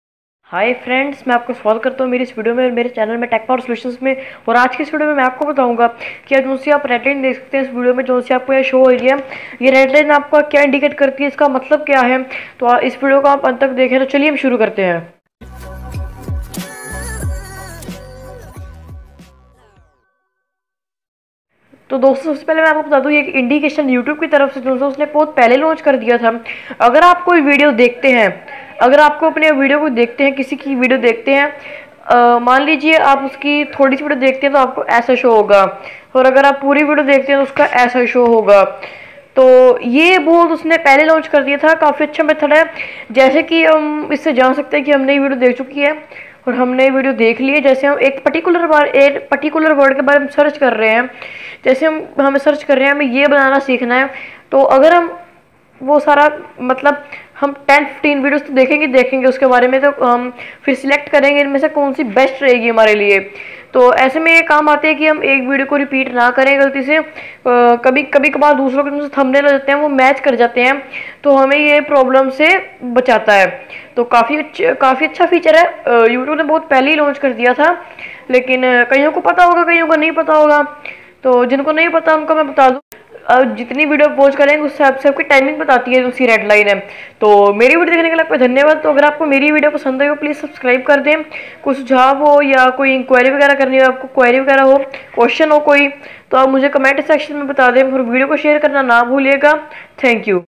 What is the meaning of red line below the video
Hi friends in this video i will tell you What is the meaning of red line below the video.